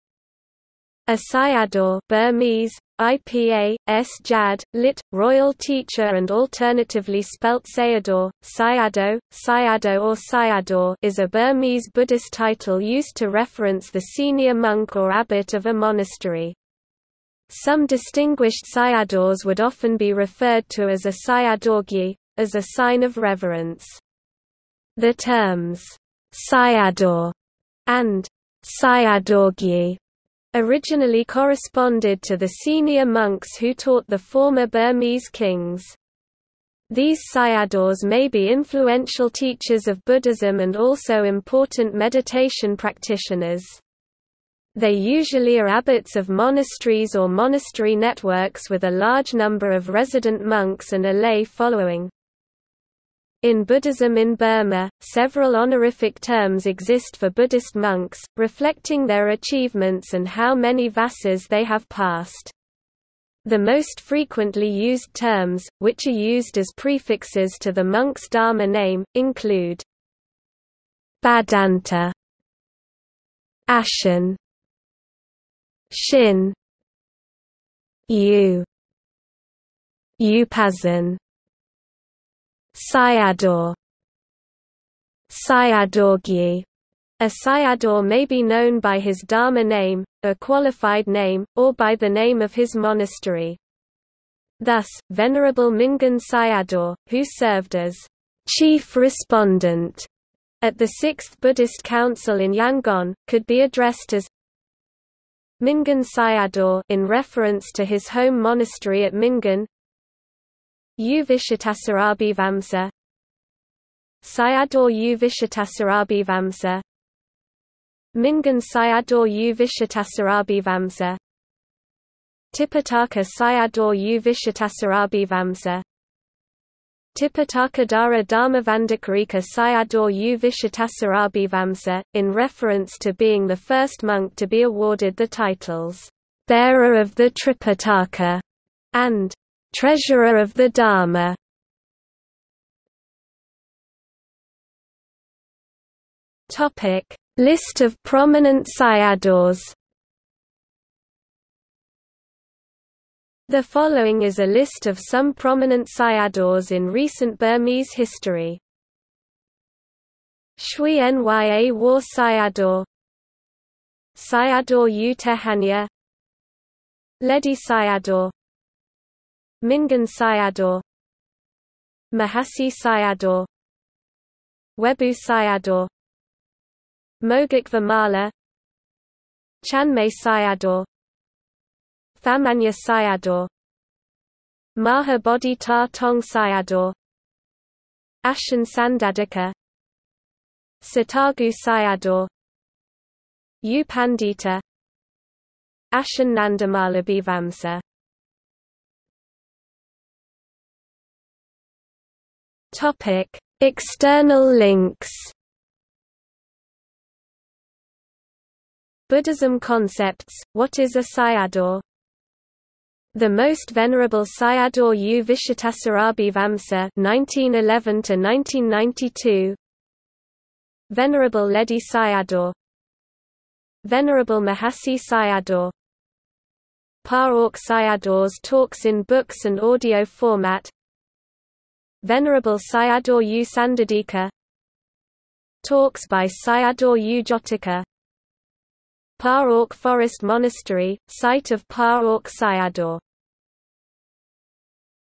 Sayadaw | Wikipedia audio article
00:02:22 List of prominent Sayadaws
00:03:19 External links

- Socrates

SUMMARY
A sayadaw (Burmese: ဆရာတော်, IPA: [sʰəjàdɔ̀]; lit. royal teacher and alternatively spelt hsayadaw, sayado, sayāḍo or sayāḍaw) is a Burmese Buddhist title used to reference the senior monk or abbot of a monastery. Some distinguished sayadaws would often be referred to as a sayadawgyi (ဆရာတော်ကြီး, as a sign of reverence. The terms "sayadaw" and "sayadawgyi" originally corresponded to the senior monks who taught the former Burmese kings.  These sayadaws may be influential teachers of Buddhism and also important meditation practitioners. They usually are abbots of monasteries or monastery networks with a large number of resident monks and a lay following.
In Buddhism in Burma, several honorific terms exist for Buddhist monks, reflecting their achievements and how many vassas they have passed. The most frequently used terms, which are used as prefixes to the monks' dharma name, include:

"Bhaddanta"
"Ashin"
"Shin"
"U"
"Upazin"
"Sayadaw"
"Sayadawgyi"A sayadaw may be known by his dharma name (ဘွဲ့), a qualified name, or by the name of his monastery. Thus, venerable Mingun Sayadaw, who served as "Chief Respondent" at the Sixth Buddhist council in Yangon, could be addressed as:

Mingun Sayadaw (in reference to his home monastery at Mingun)
U Vicittasarabhivamsa
Sayadaw U Vicittasarabhivamsa
Mingun Sayadaw U Vicittasarabhivamsa
Tipitaka Sayadaw U Vicittasarabhivamsa
Tipitakadhara Dhammabhandakarika Sayadaw U Vicittasarabhivamsa, in reference to being the first monk to be awarded the titles "Bearer of the Tripiṭaka" and "Treasurer of the Dhamma"